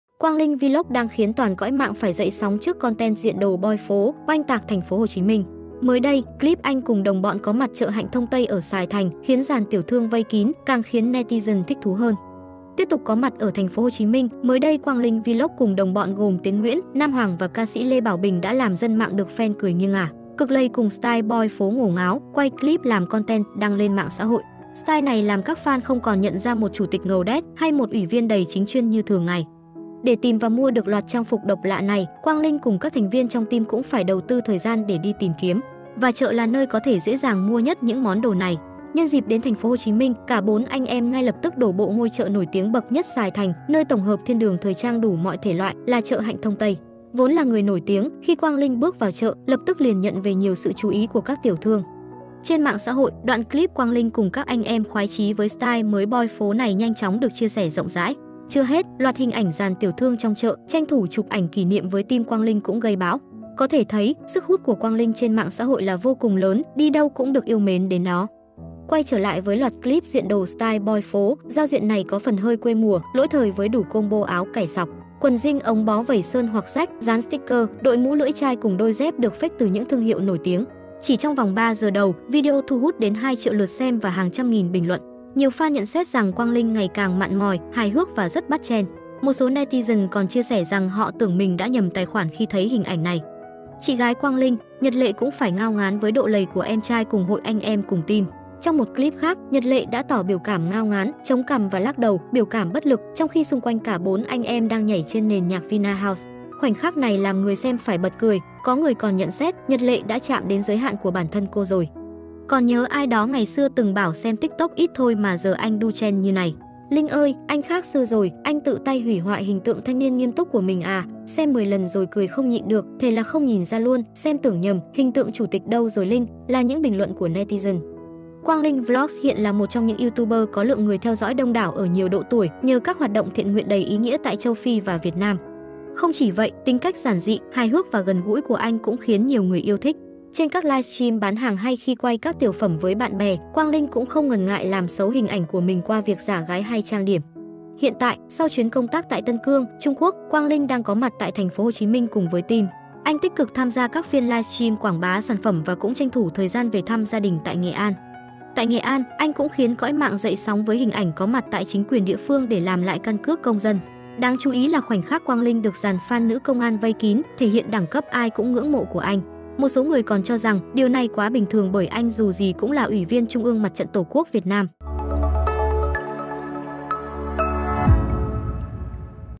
Quang Linh đi chợ ở TPHCM, dàn tiểu thương rần rần
Quang Linh Vlog đang khiến toàn cõi mạng phải dậy sóng trước content diện đồ boy phố, oanh tạc TPHCM. Mới đây, clip anh cùng “đồng bọn” có mặt chợ Hạnh Thông Tây ở Sài thành, khiến dàn tiểu thương vây kín, càng khiến netizen thích thú hơn.

Tiếp tục có mặt ở TPHCM, mới đây Quang Linh Vlog cùng “đồng bọn” gồm Tiến Nguyễn, Nam Hoàng và ca sĩ Lê Bảo Bình đã làm dân mạng được phen cười nghiêng ngả, cực lầy cùng style boy phố ngổ ngáo, quay clip làm content đăng lên mạng xã hội. Style này làm các fan không còn nhận ra một chủ tịch ngầu đét, hay một Uỷ viên đầy chính chuyên như thường ngày.

Để tìm và mua được loạt trang phục độc lạ này, Quang Linh cùng các thành viên trong team cũng phải đầu tư thời gian để đi tìm kiếm. Và chợ là nơi có thể dễ dàng mua nhất những món đồ này. Nhân dịp đến TPHCM, cả 4 anh em ngay lập tức đổ bộ ngôi chợ nổi tiếng bậc nhất Sài thành, nơi tổng hợp thiên đường thời trang đủ mọi thể loại, là chợ Hạnh Thông Tây. Vốn là người nổi tiếng, khi Quang Linh bước vào chợ, lập tức liền nhận về nhiều sự chú ý của các tiểu thương.